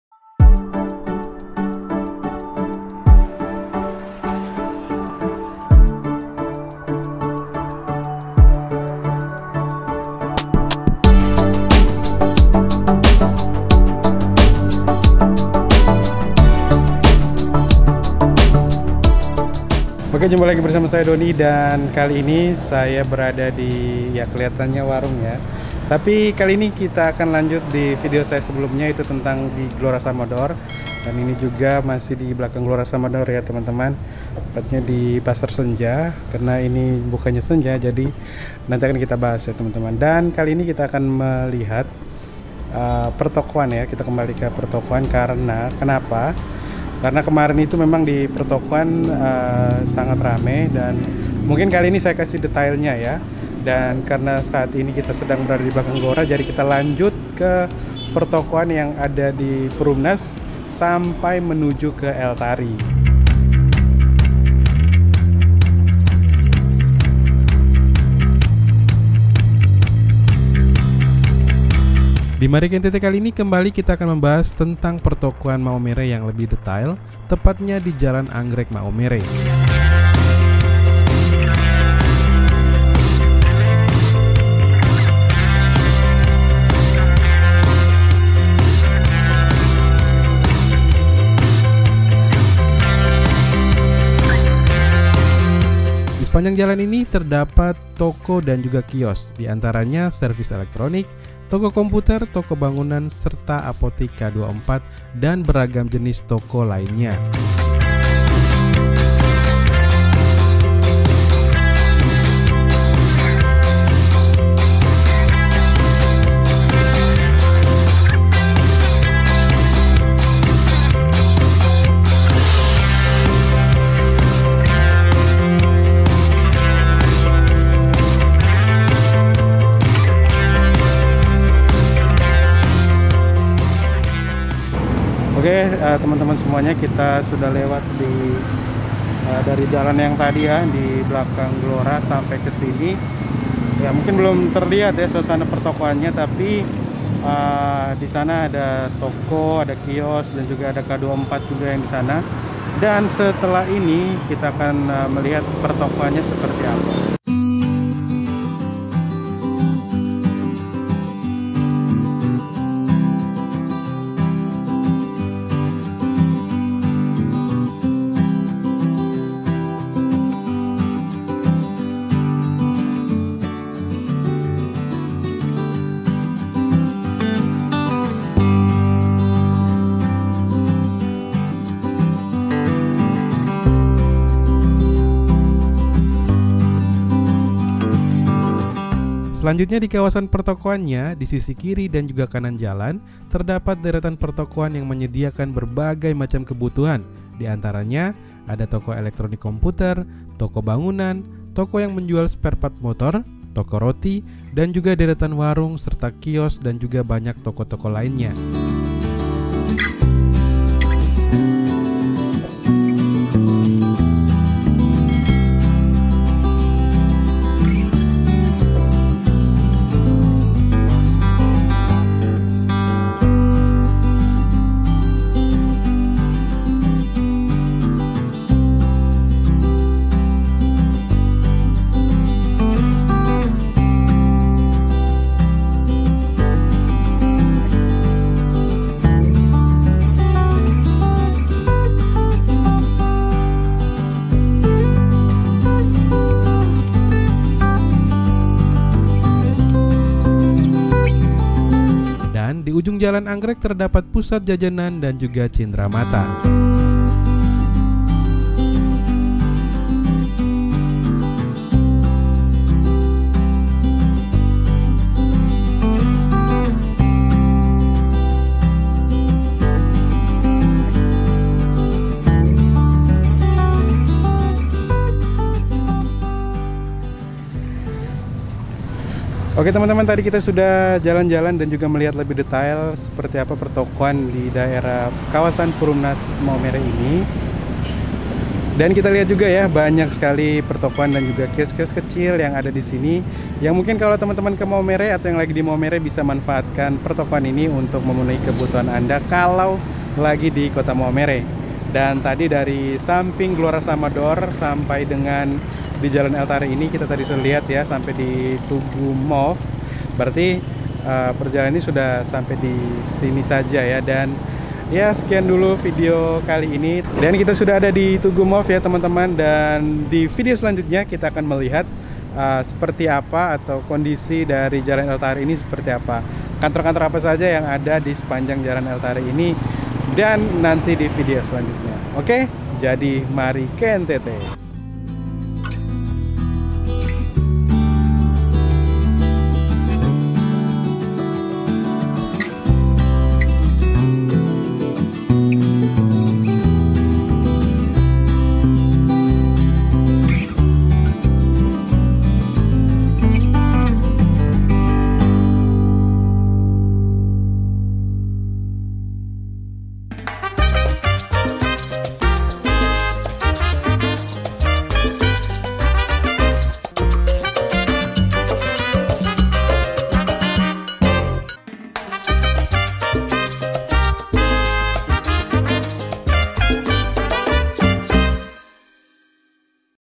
Kawasan Pertokoan Maumere Di Jl. Anggrek Maumere, Seramai Apa Sih Toko -Tokonya ? | Mari Ke NTT
Nusa Tenggara Timur ( NTT ) Memeliki keanekaragaman Wisata Dan Budaya, Yukk Untuk teman - teman silahkan menjelajah Nusa Tenggara Timur